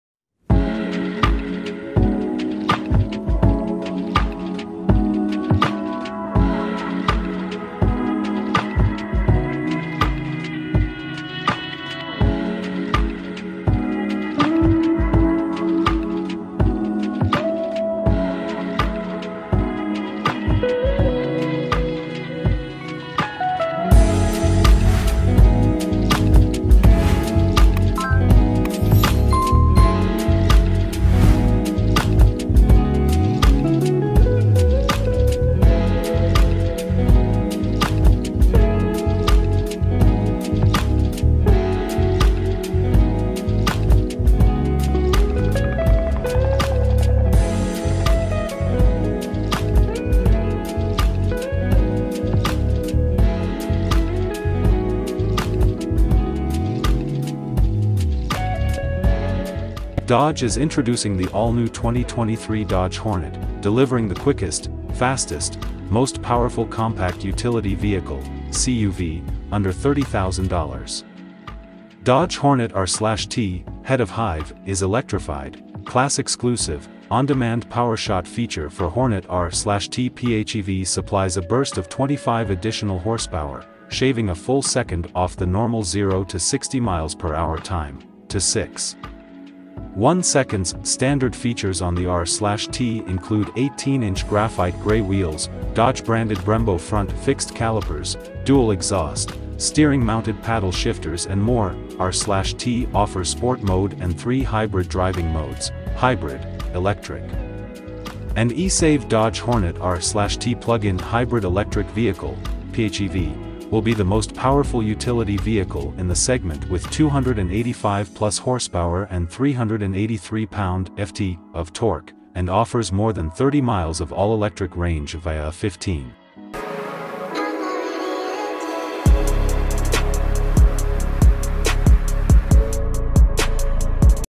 2023 All New Dodge Hornet R/T PHEV - Review
DODGE HORNET R/T PHEV 2023

Dodge is poised to get back into the small crossover market with the 2023 Hornet. Rather than reskinning a Jeep Compass like it did with the unloved Caliber, this time around Dodge has instead settled on sharing a platform with a Stellantis-company cousin, the Alfa Romeo Tonale. A 265-hp turbocharged four-cylinder with all-wheel drive and a nine-speed automatic is the standard setup but the R/T model is a plug-in hybrid with 285 ponies and more than 30 miles of electric driving. Not only will the Hornet offer Dodge’s first hybrid powertrain it’ll also boast the most high-tech equipment in the brand’s lineup, with standard features that include a digital gauge display, Uconnect 5 infotainment software, and automated emergency braking with pedestrian-and-cyclist detection. The nonhybrid GT models are expected to go on sale by the end of 2022 with the R/T PHEV models following shortly after in early 2023.

The Hornet will be a new entrant in the Dodge lineup and will sit below the mid-size Durango. This small SUV will compete against rivals such as the compact Kia Seltos, the Mazda CX-30, and the Volkswagen Taos.

Dodge is offering the Hornet as either the GT or R/T PHEV for the 2023 model year, although other trims could be added later on. Dodge says the entry-level GT model will start around $30,000. When we find out more about the Hornet’s pricing and options, we’ll update this story with details and a recommendation on which model offers the most value.

Hornet GT models are powered by a turbocharged 2.0-liter four-cylinder that makes 265 horsepower and comes paired with a nine-speed automatic transmission. All-wheel drive is optional, and Dodge says this powertrain is good enough to motivate the Hornet to 60 mph in 6.5 seconds. The R/T model is Dodge’s first plug-in hybrid and it’s powered by a turbocharged 1.3-liter four-cylinder and an electric motor that together combine for 285 horsepower. Instead of the nine-speed automatic from the GT, the R/T gets a six-speed automatic but still comes standard with all-wheel drive. The powertrain can temporarily boost output by 25 hp via a feature called PowerShot that’s activated by pulling both paddle shifters. With the PowerShot mode engaged, Dodge says the R/T PHEV can hit 60 mph in 6.1 seconds. When we get a chance to test these claims for ourselves, we’ll update this story with the Hornet’s performance results and driving impressions.